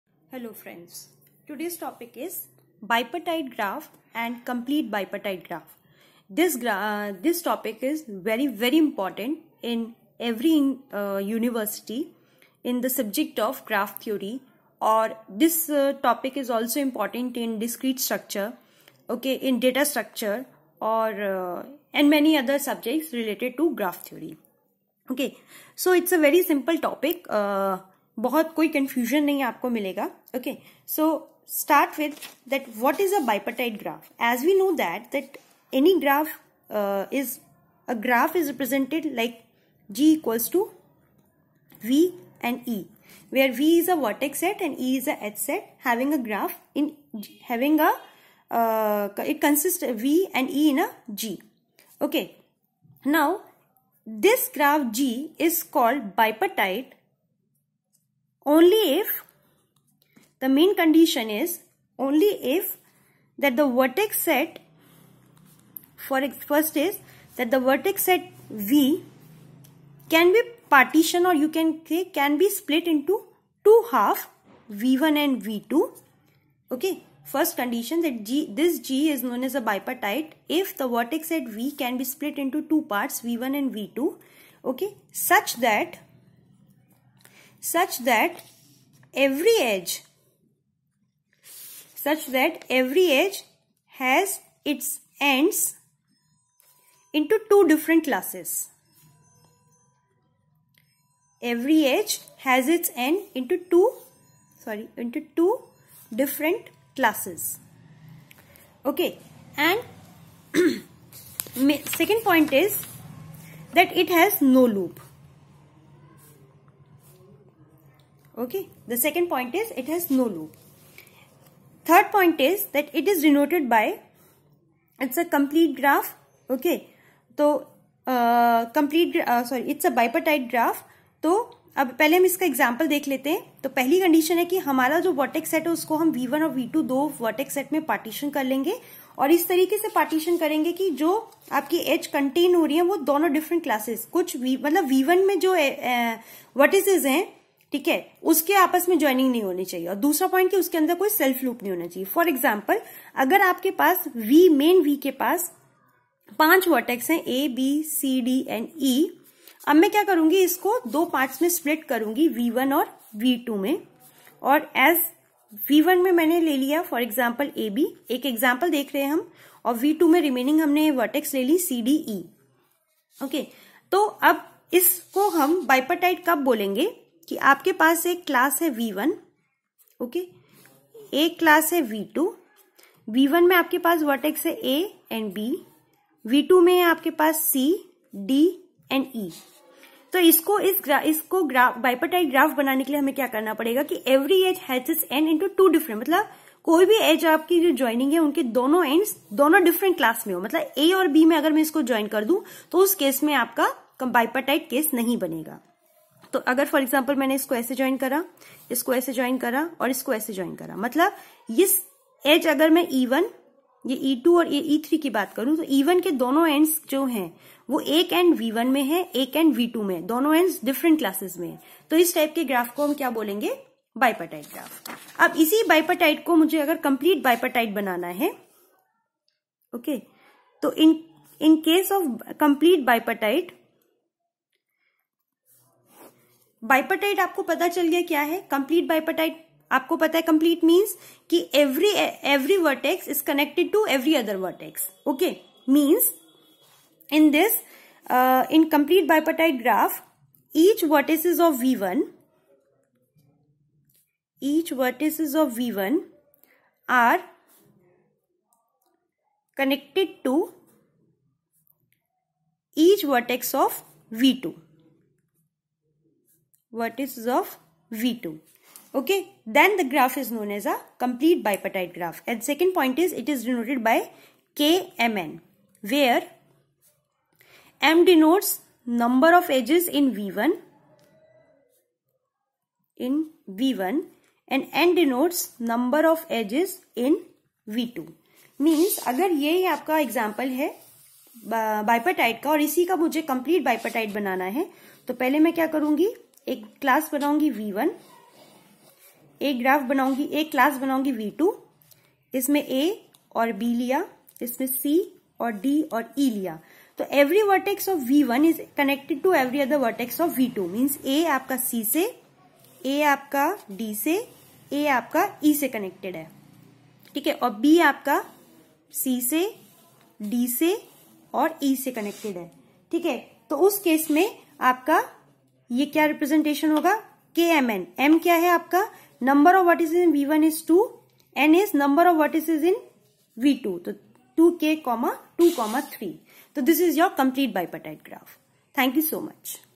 2(A). Bipartite and Complete Bipartite Graph
In this lecture we are discussing the concepts of Bipartite and Complete Bipartite Graphs with examples.